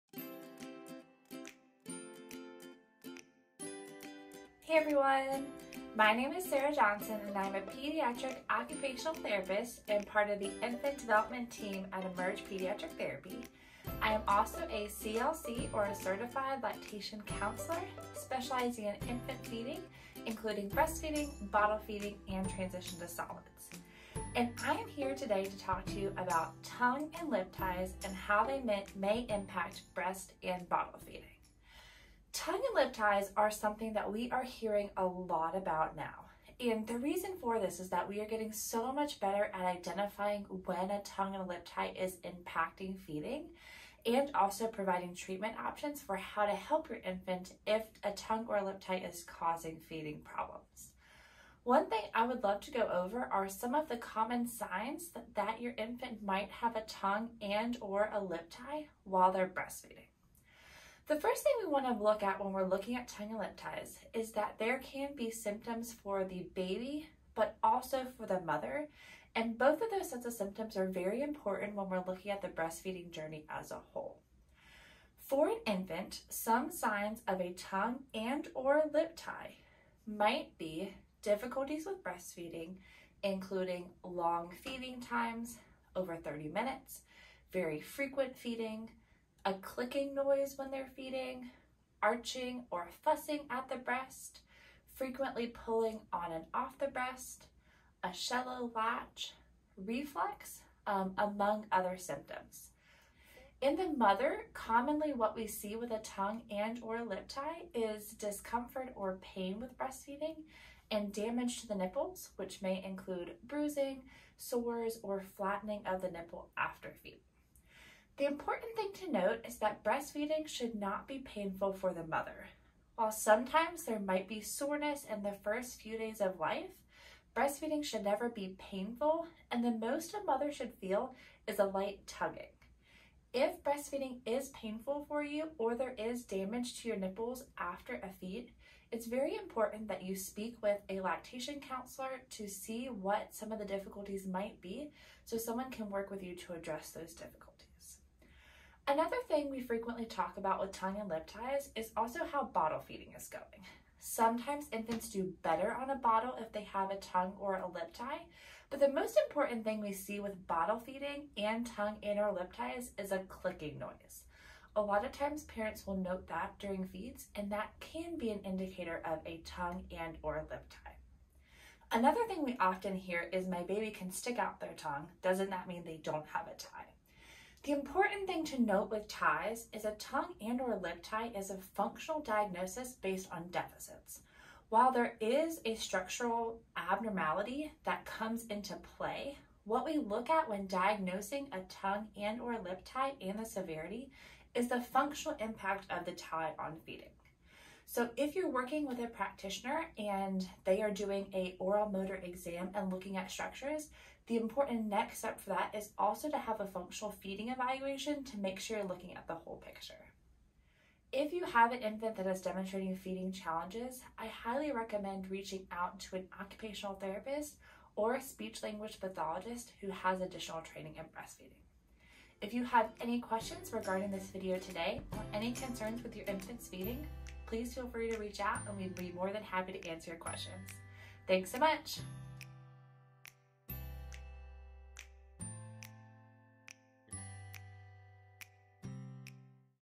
Tongue & Lip Ties: what to look for when feeding
In this video, Occupational Therapist and Lactation Counselor Dr. Sarah describes common signs of tongue/lip ties to look for when breastfeeding or bottle feeding. Breastfeeding may hurt in the first few days of life, but if you're experiencing ongoing pain, reach out to a Certified Lactation Counselor (CLC) for help!